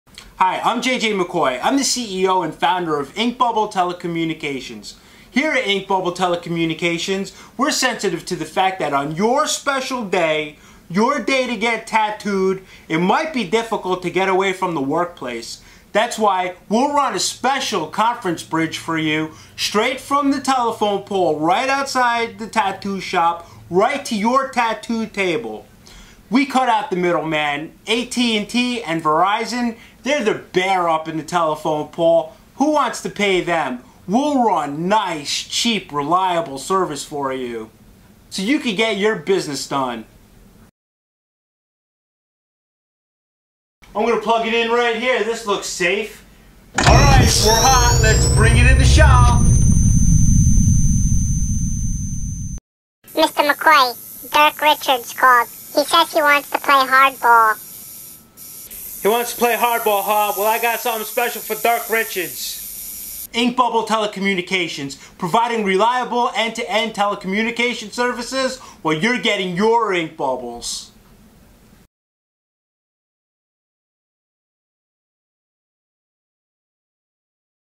Ink Bubbles Telecommunication - Providing Telecom Service While Getting your Tattoo
Video Class Assignment -  Fall Semester 2015 - Combine a random business name and random business description and create a short video using this criteria.